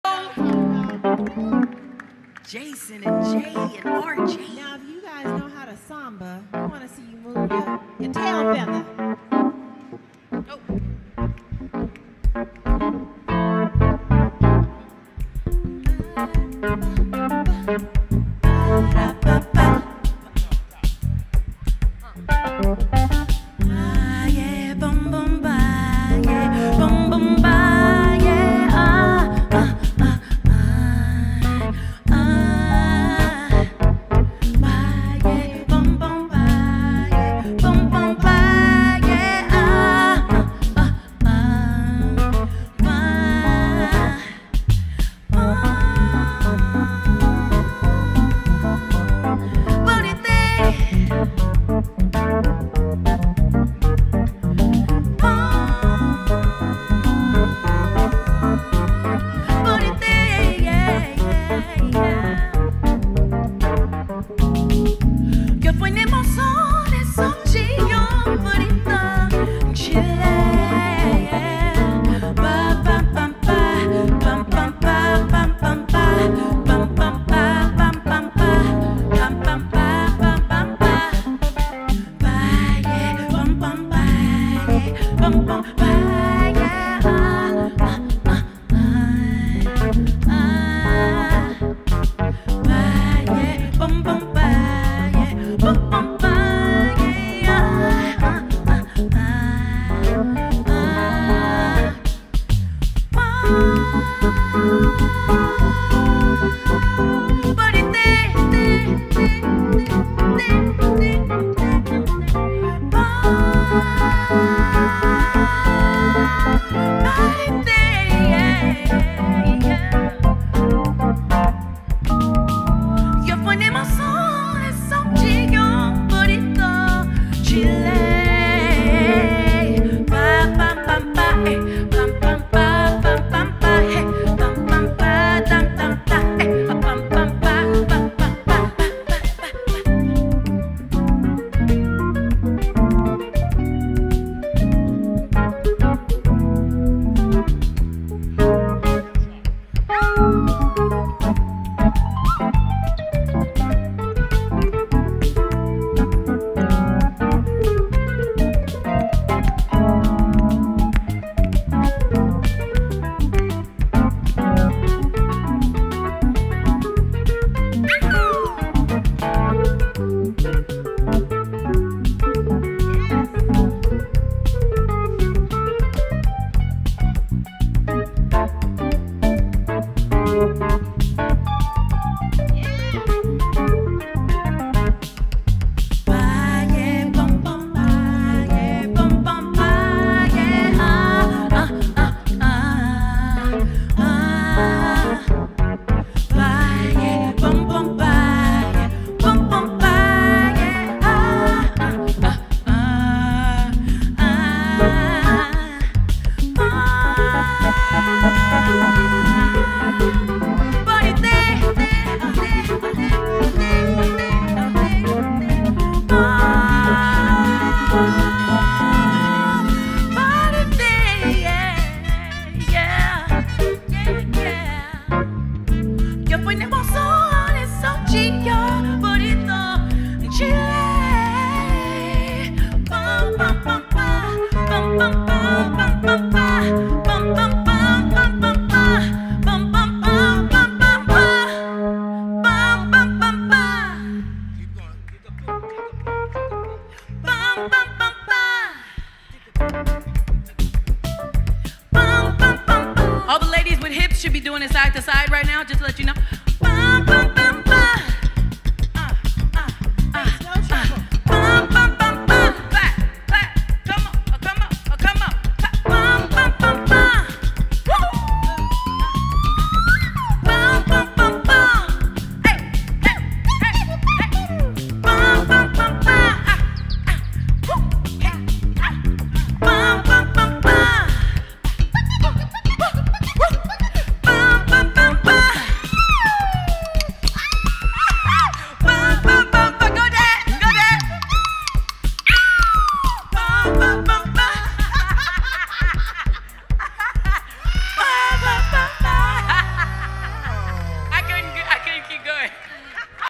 Studiologic at NAMM 2024: Wayne Vaughn PART 3/4
Check out the phenomenal Wayne Vaughn and friends performing live at the Studiologic Booth at NAMM Show 2024.